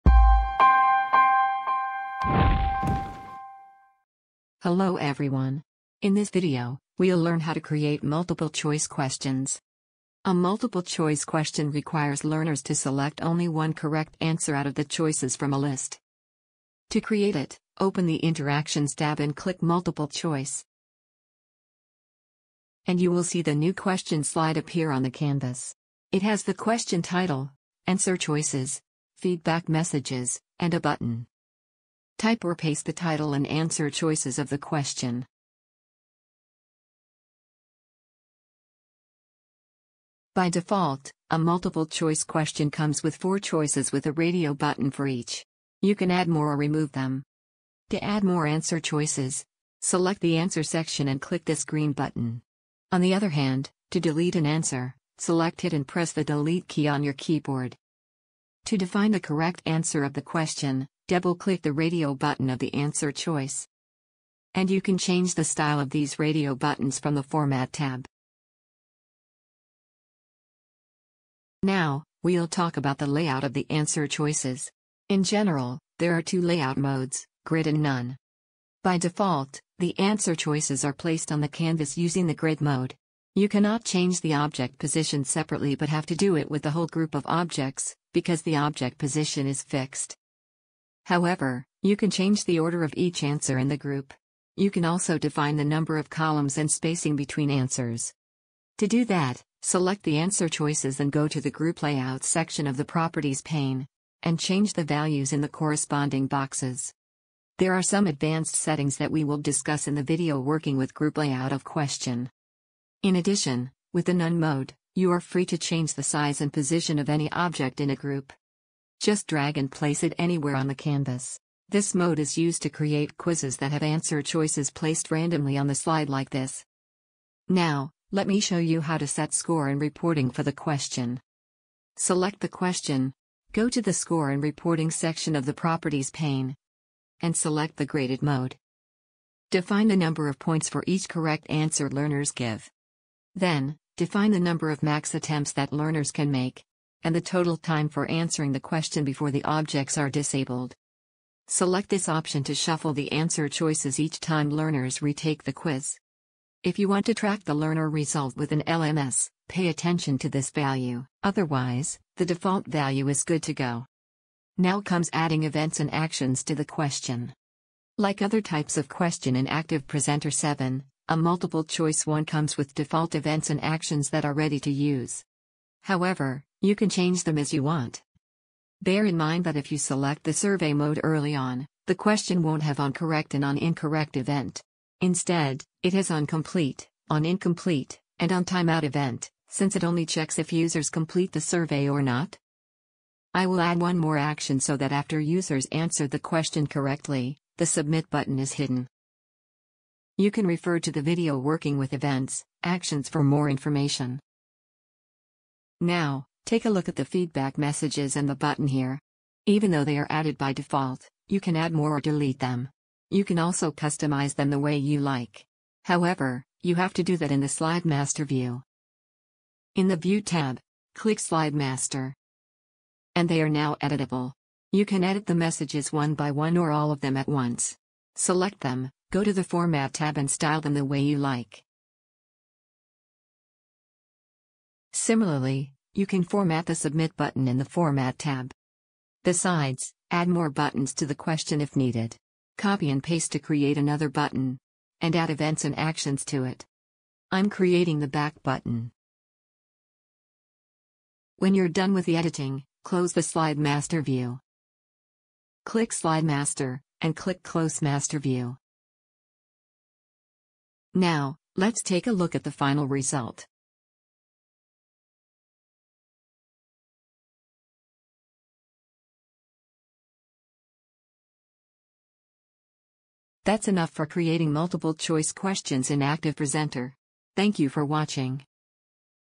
Creating Multiple Choice Questions - ActivePresenter 7
In this video, we’ll learn how to create multiple choice questions. It’s a type of questions that requires learners to select only one correct answer out of the choices from a list.